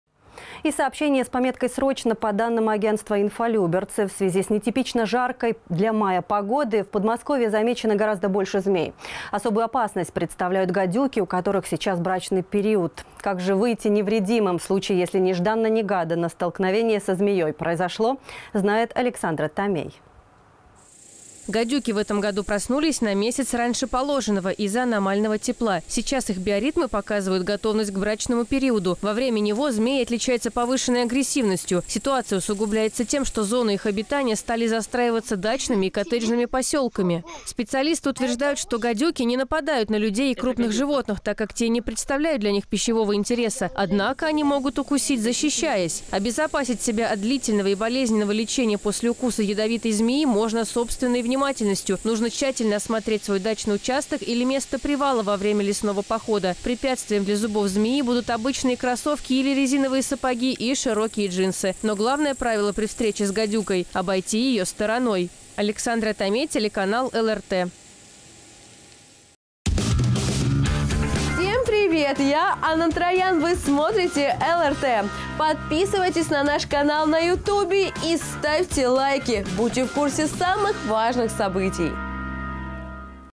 В Московской области проснулись гадюки
Под воздействием нехарактерно жаркой для мая погоды в Подмосковье стало гораздо больше змей, чем обычно. Опасность представляют гадюки, у которых сейчас брачный период. Об этом сообщает агентство «РИАМО». Как обезопасить себя от встречи с пресмыкающимися расскажет Александра Томей.

Гадюки в этом году проснулись на месяц раньше положенного из-за аномального тепла. Сейчас их биоритмы показывают готовность к брачному периоду. Во время него змеи отличаются повышенной агрессивностью. Ситуация усугубляется тем, что зоны их обитания стали застраиваться дачными и коттеджными поселками.
Специалисты утверждают, что гадюки не нападают на людей и крупных животных, так как те не представляют для них пищевого интереса. Однако они могут укусить, защищаясь.
Обезопасить себя от длительного и болезненного лечения после укуса ядовитой змеи можно собственной внимательностью. Нужно тщательно осмотреть свой дачный участок или место привала во время лесного похода. Препятствием для зубов змеи будут обычные кроссовки или резиновые сапоги и широкие джинсы. Но главное правило при встрече с гадюкой – обойти ее стороной.

Александра Томей